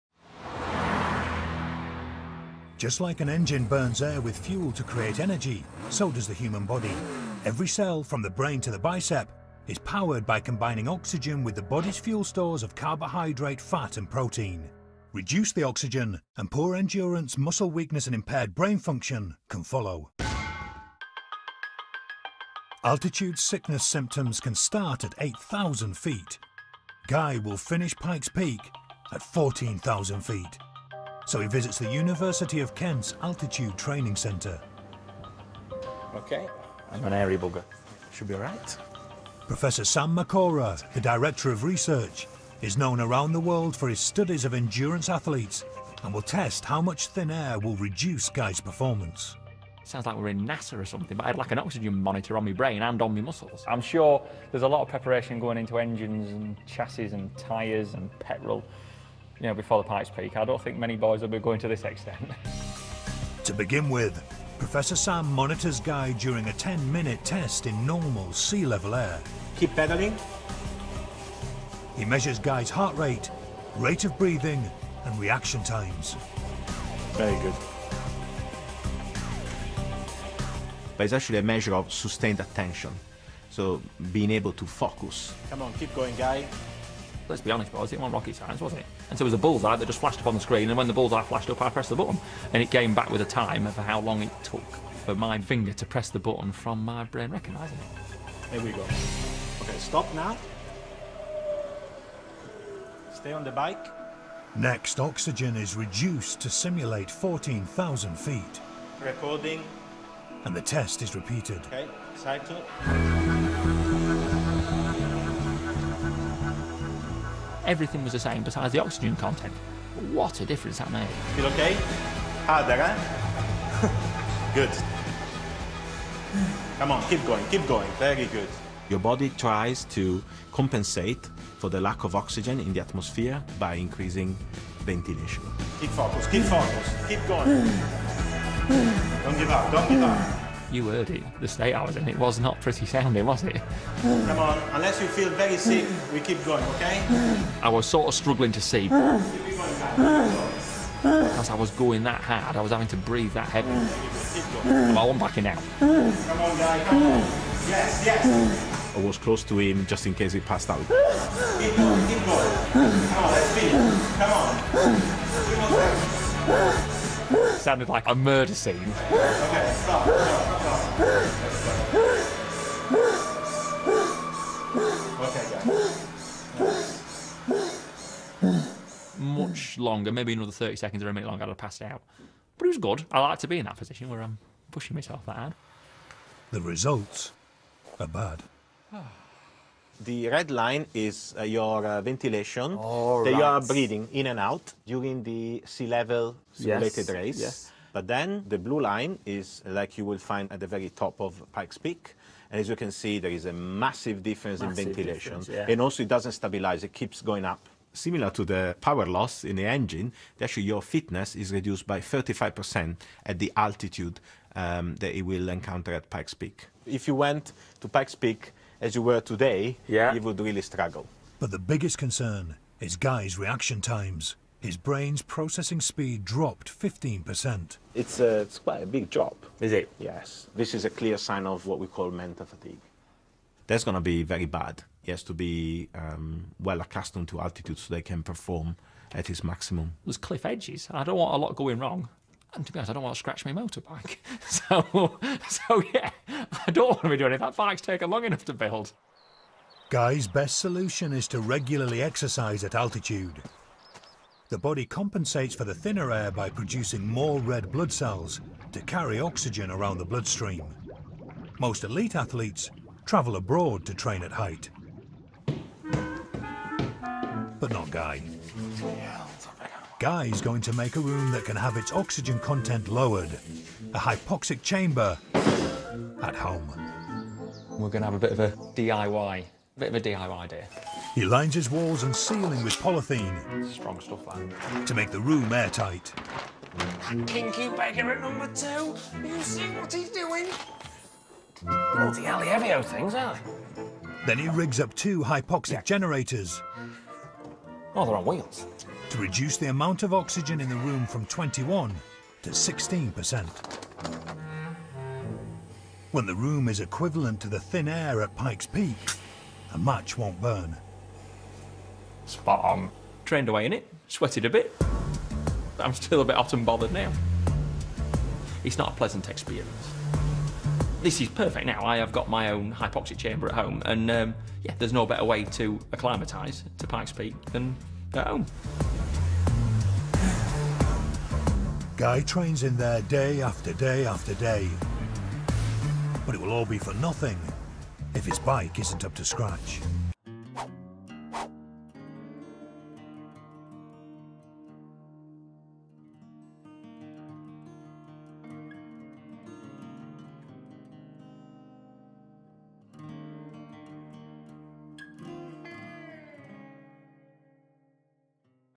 Guy's Altitude Training | Guy Martin Proper
Guy visits the USA, where he hopes to win at one of America's oldest races: the Pike's Peak International Hill Climb.